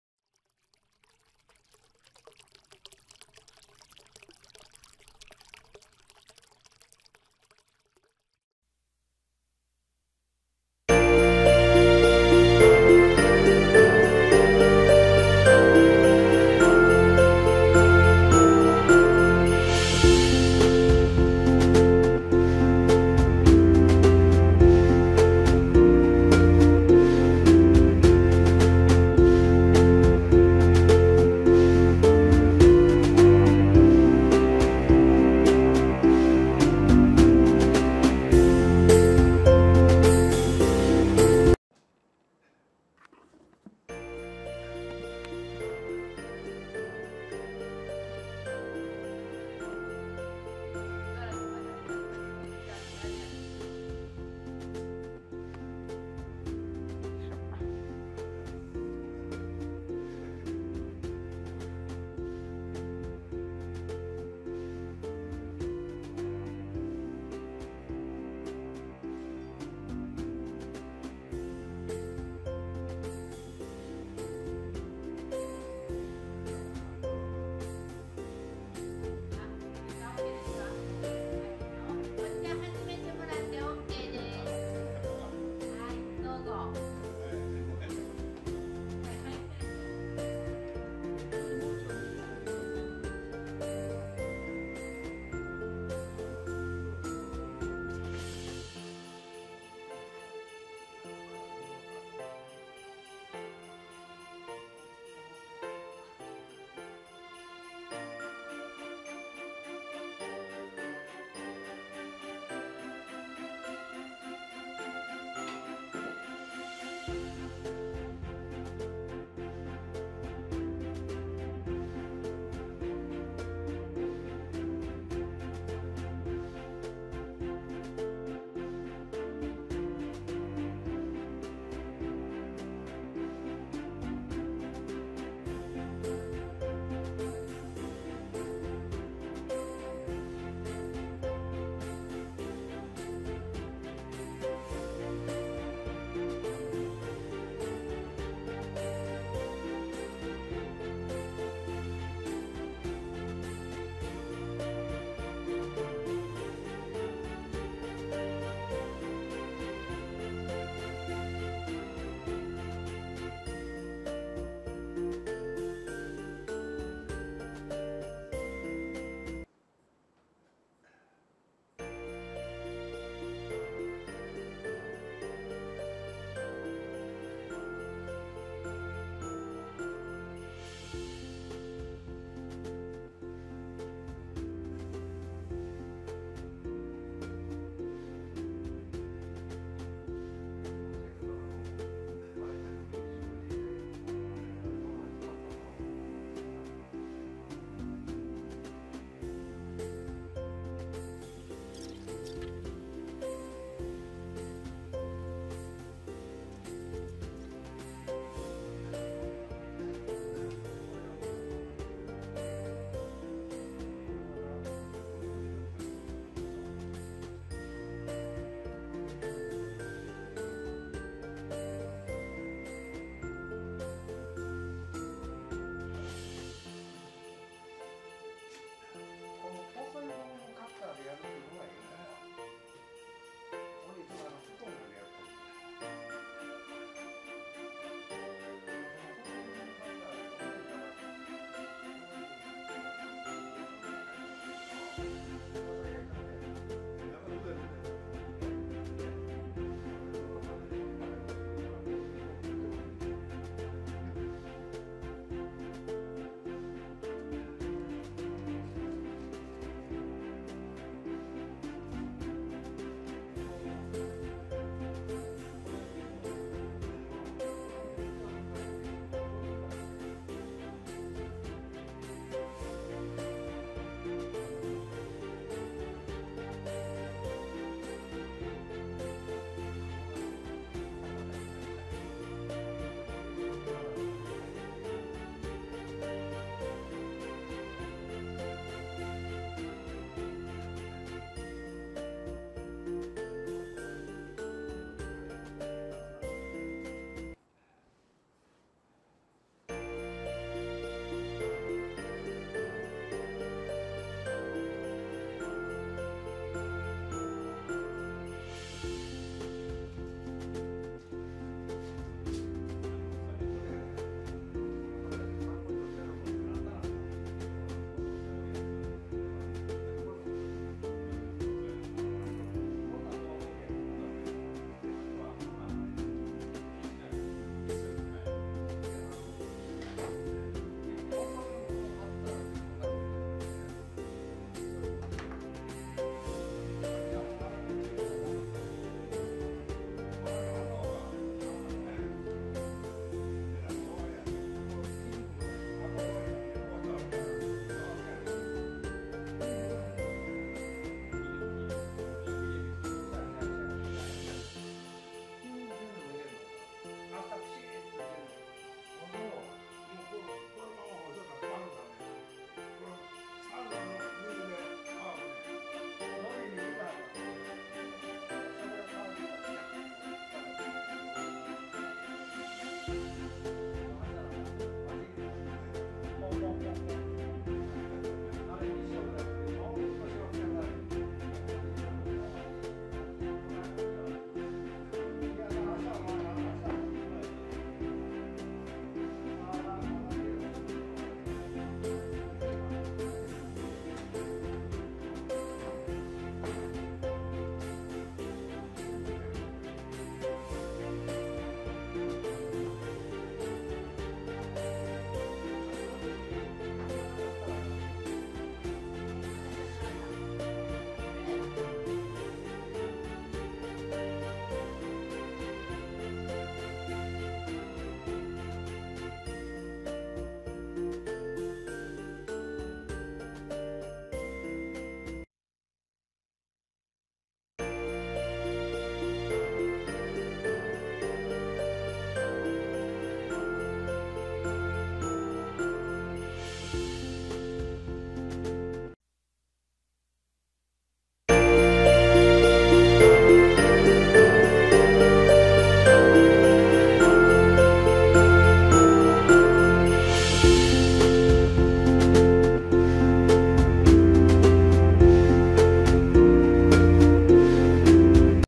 障子貼り Paper shoji pasting
障子貼りは結構手間がかかる。
障子貼り名人９０歳。

１枚貼るのに３分。
コツは糊の適度な硬さと切る時のカッターによる。

It takes a lot of time to put up the shoji screens.
A 90-year-old shoji-pasting expert.

It takes 3 minutes to apply one sheet.
The trick is to use a moderate hardness of glue and a cutter when cutting.

設備屋の適当田舎暮らし